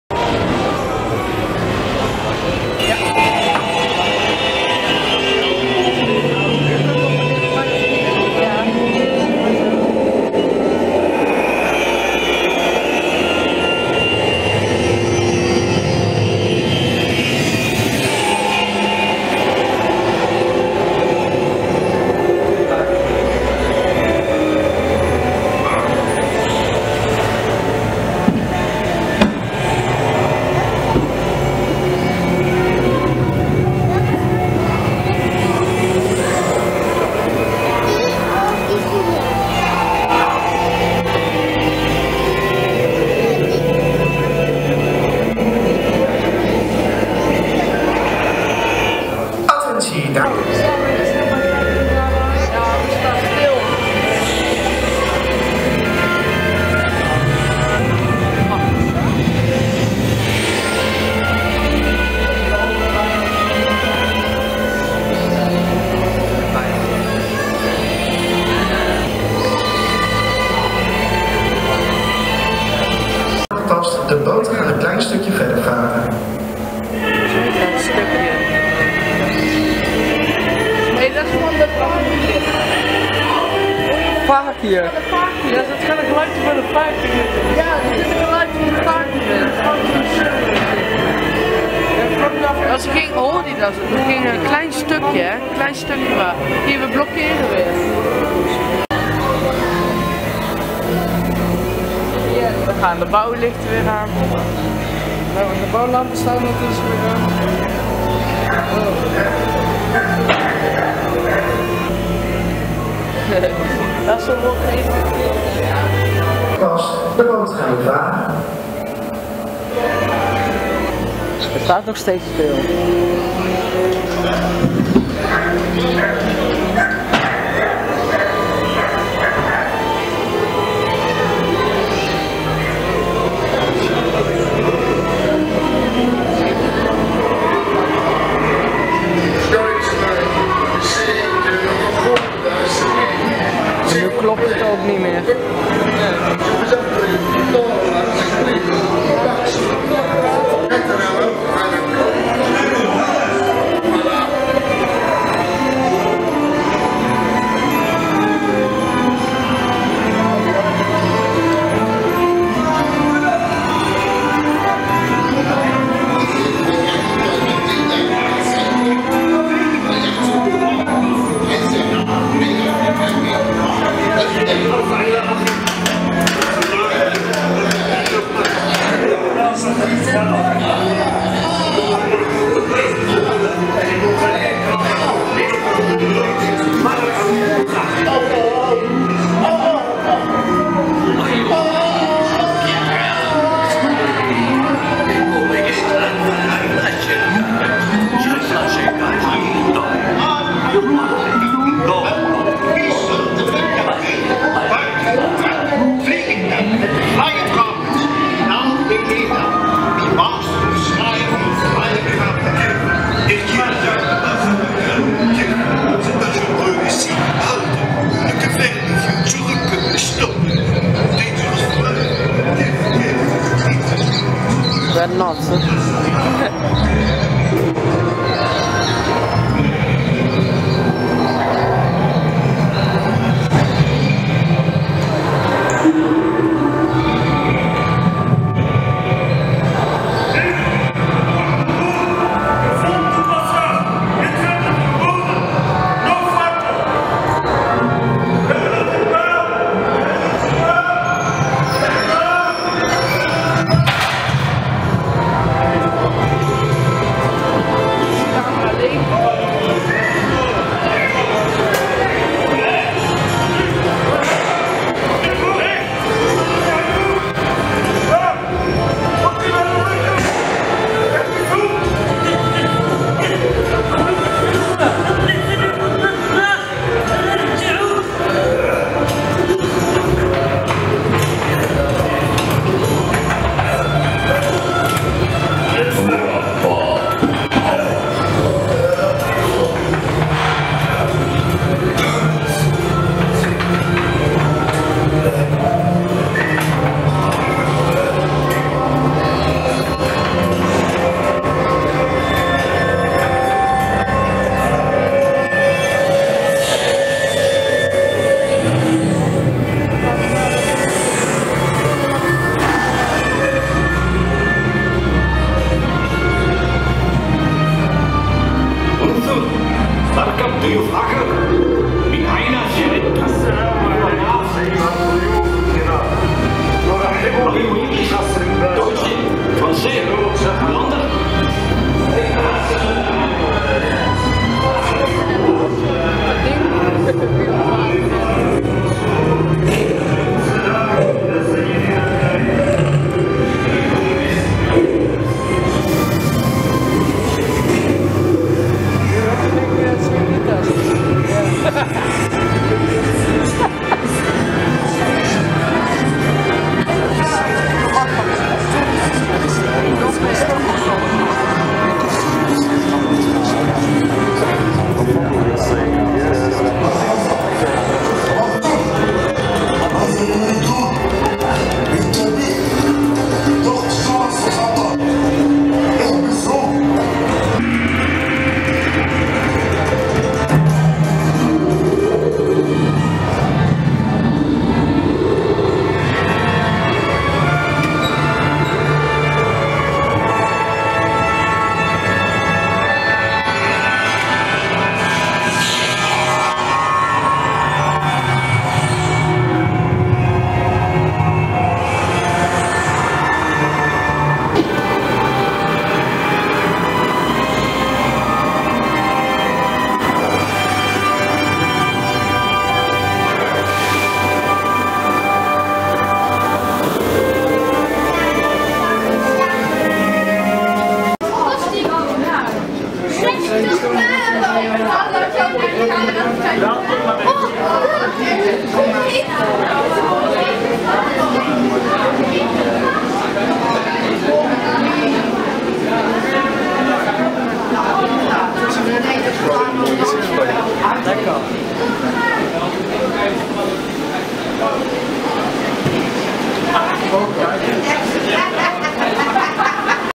Fata Morgana Onride met storing en fakir geluid - @Efteling 18-02-2018
Onride van Fata Morgana in de Efteling, Samen met Mikey en Tjalle.
We hadden een storing in de jungle.
Na eventjes wachten hoorden we het geluid van de fakir.